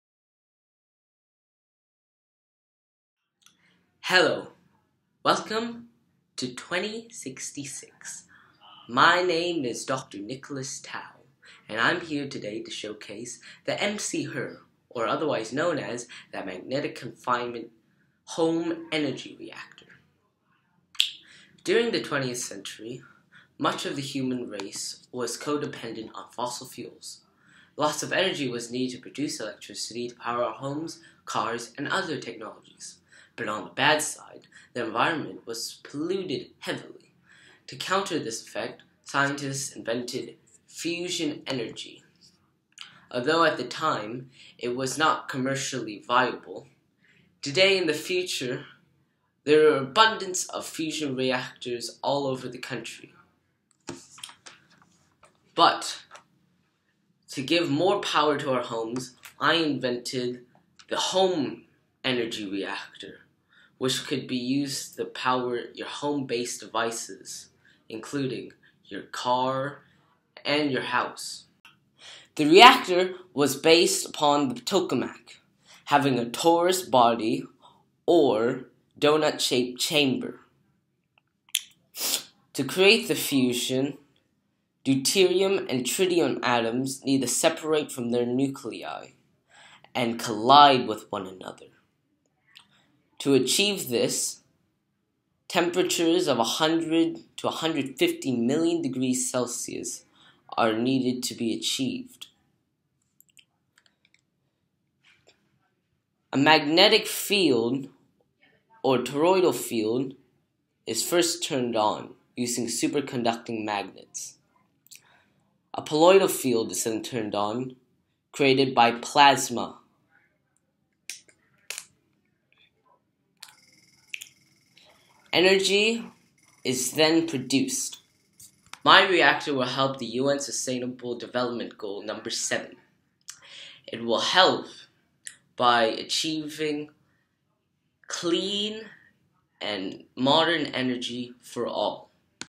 Fusion Magnetic Confinement Home Energy Reactor
In the year 2066, Fusion reactors as become the main source for electrical energy. To produce more electricity to power this futures exciting technologies, the MC-HER is a small home reactor. This reactor will accomplish the United Nation's Goal 7: Ensure access to affordable, reliable, sustainable and modern energy for all.